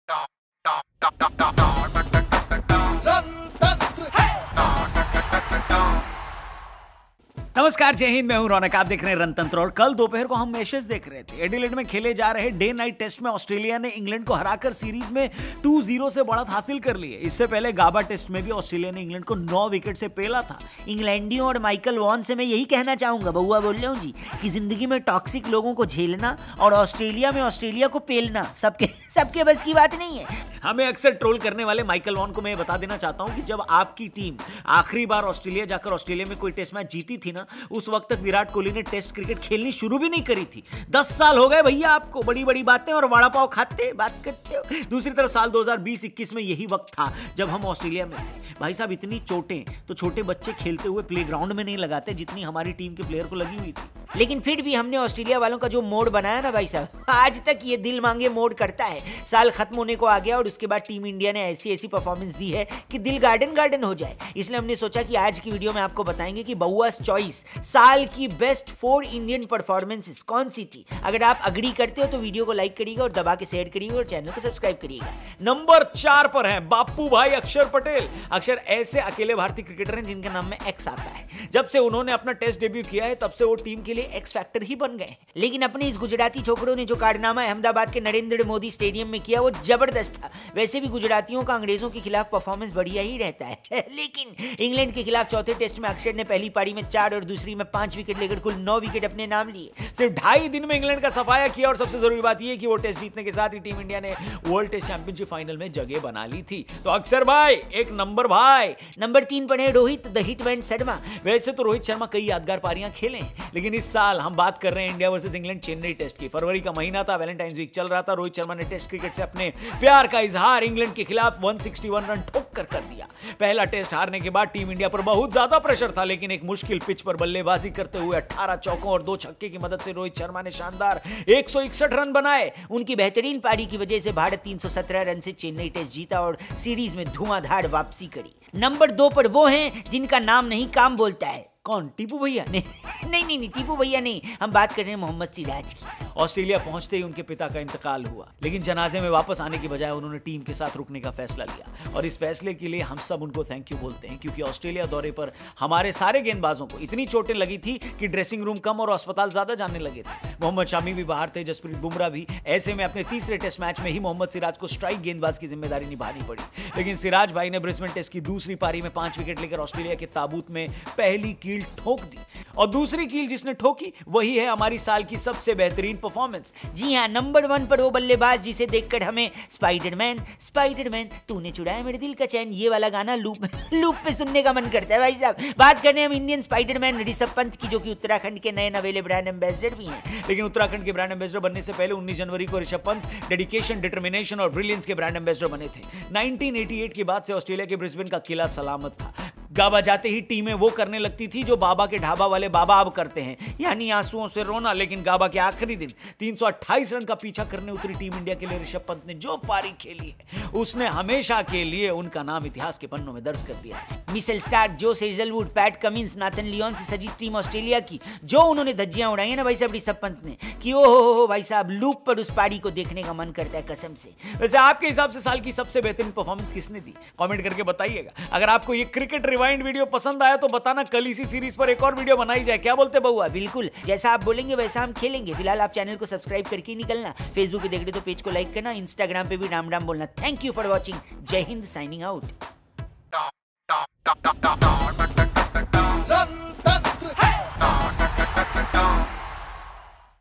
कौन है इस साल का BEST INDIAN PERFORMER? | Year 2021 at a glance | Team India | BCCI | RJ Raunak
In this year, which Indian player had performed outstandingly good for team India. let's find out those 4 BEST PERFORMING PLAYERS